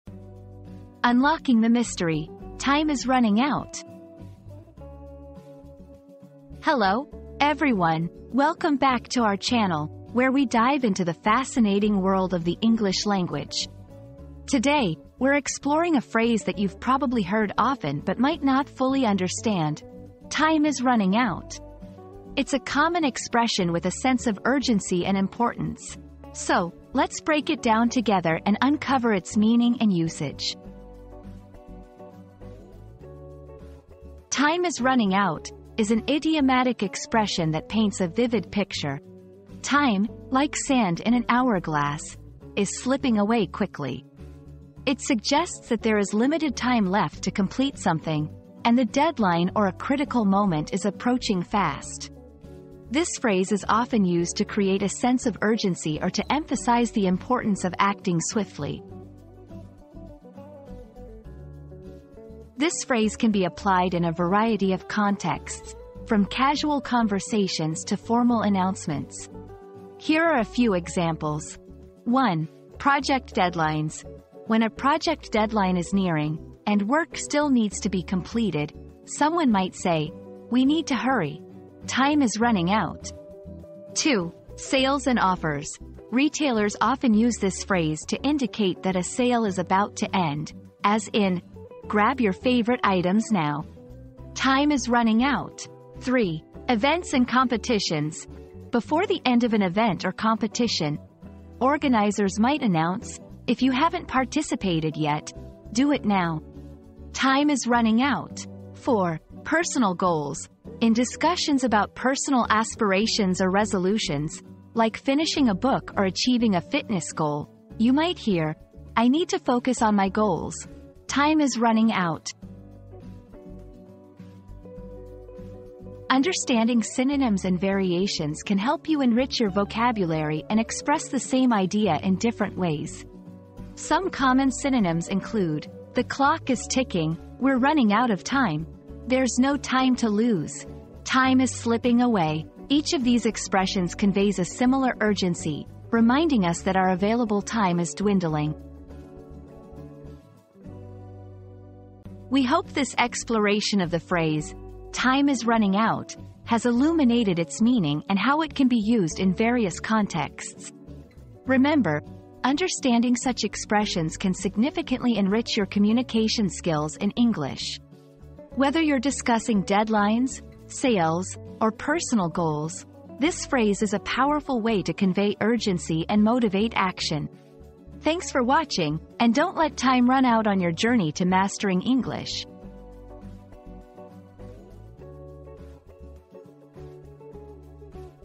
Unlocking the Mystery: "Time is Running Out"
Unraveling the Enigma: 'Time is Running Out' • Join us on a thrilling journey as we uncover the secrets behind 'Time is Running Out' and explore the urgency of the ticking clock. Don't miss out on this captivating adventure!

00:00 • Introduction - Unlocking the Mystery: "Time is Running Out"
00:33 • Understanding the Phrase
01:02 • When to Use "Time is Running Out"
02:05 • Variations and Synonyms